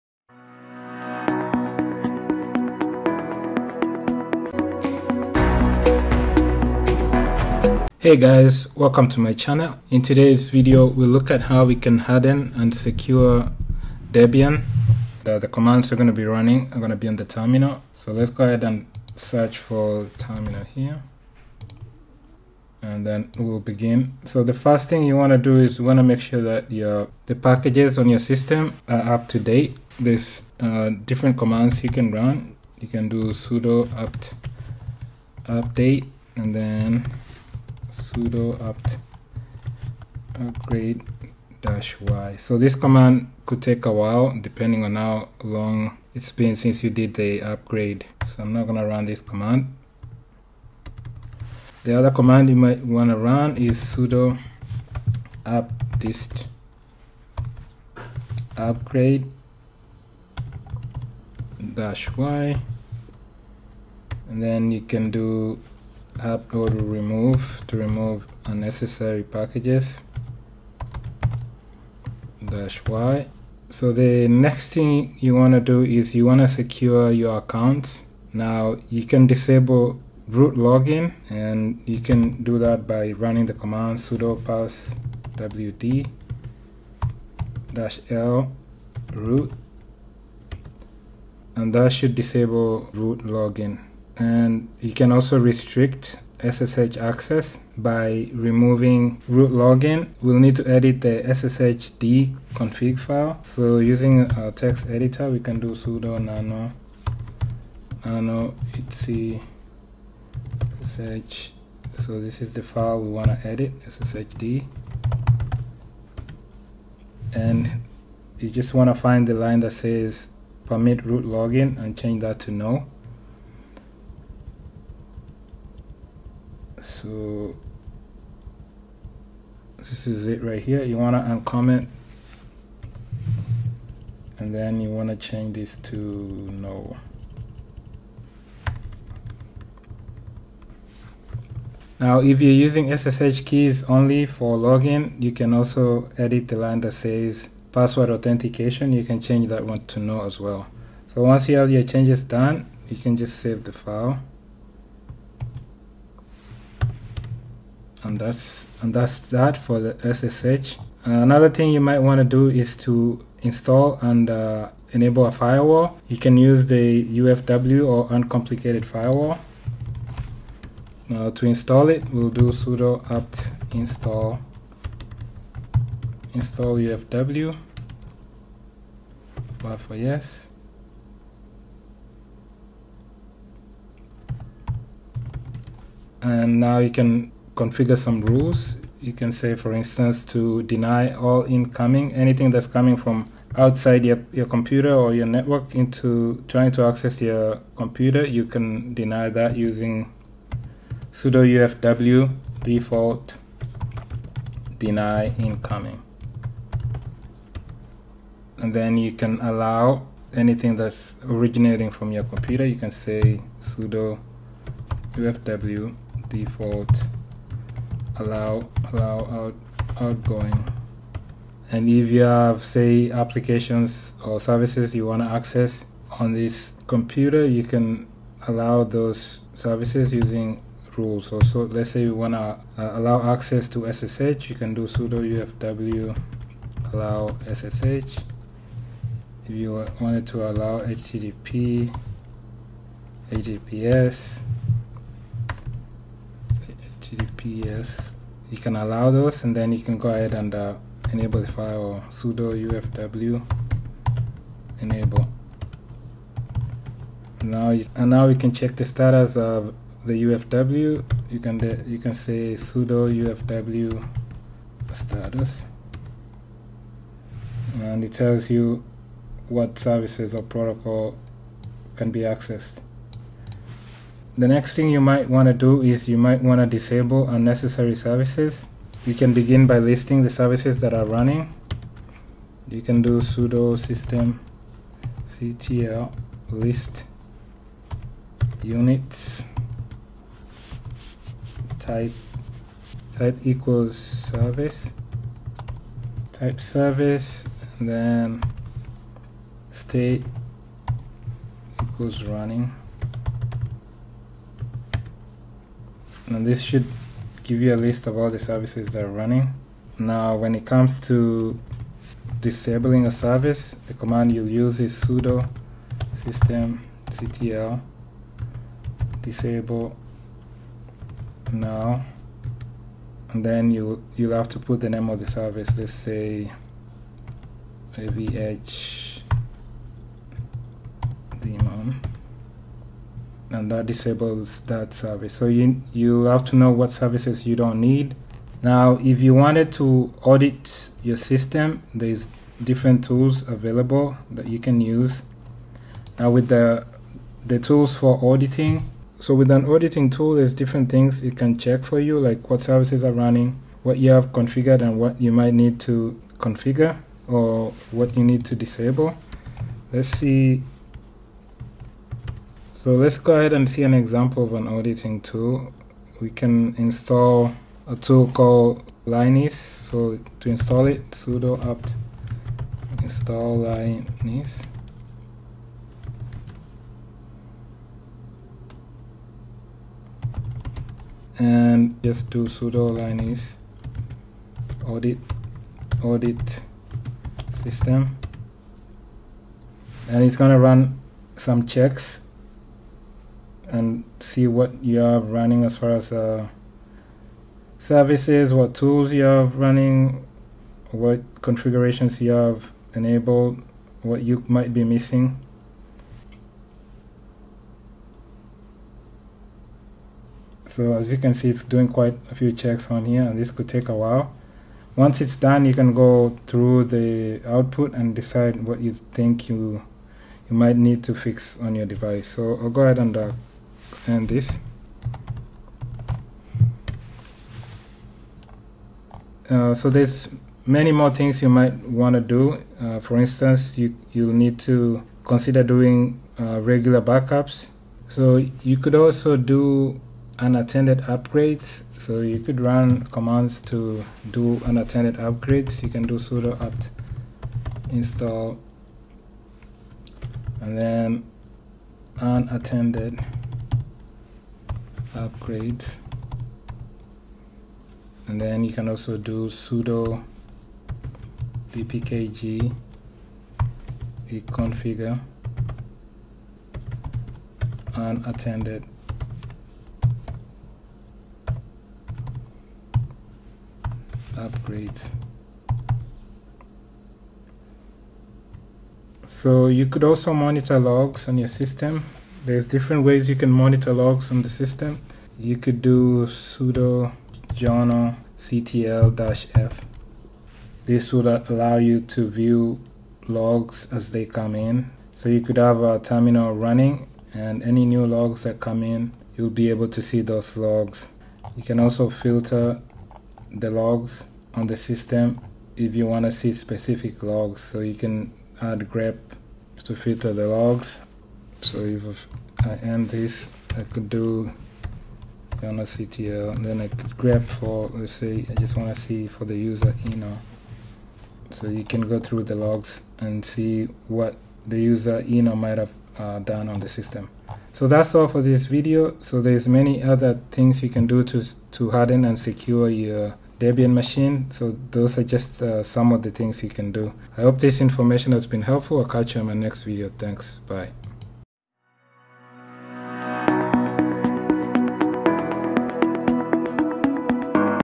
How to Harden and Secure Debian
Learn how to secure your Debian system and protect it from potential threats and vulnerabilities. In this video, we'll cover essential security best practices and techniques to harden your Debian installation, including user account management, firewall configuration, and software updates. Whether you're a beginner or an experienced Linux user, this video will provide you with the knowledge and tools you need to keep your system safe and secure.